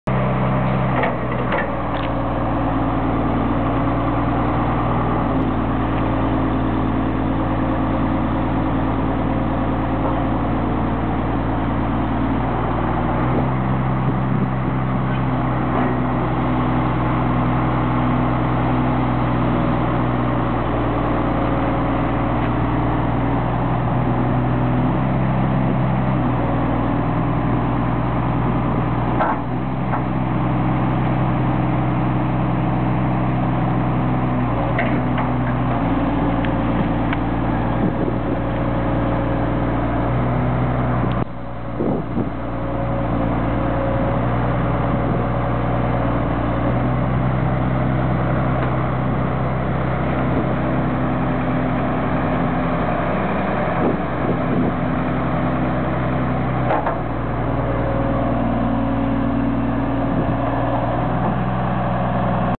1995 Caterpillar 320N Hydraulic Excavator - TRO 0321132
6 cylinder turbo diesel track excavator.  First stick is 18'8" with 9'6" dipper stick.  32" wide 5 tooth bucket.